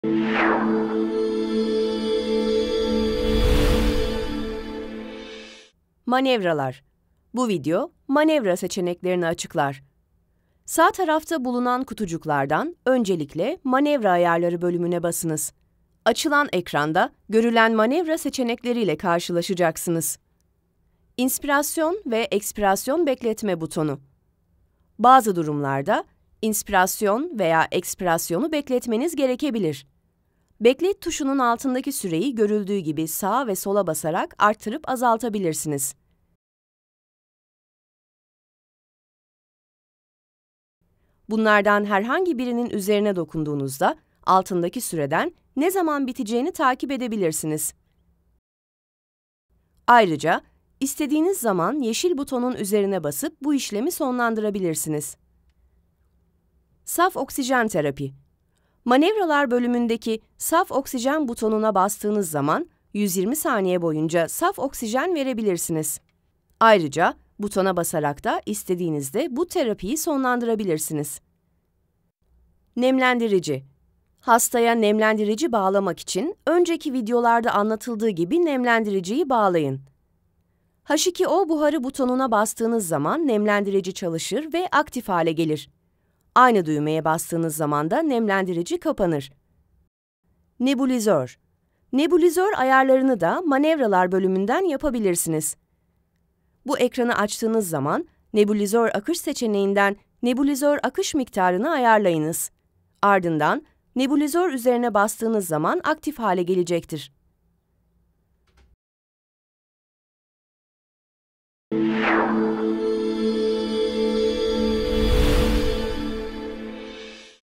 Biyovent Manevralar (Biyovent Manuevers) | #6
Bu video manevra seçeneklerini açıklar.

Video İçeriği:
00:15 - Manevra Ayarları
01:13 - Nemlendirici
01:30 - Nebülizör Ayarları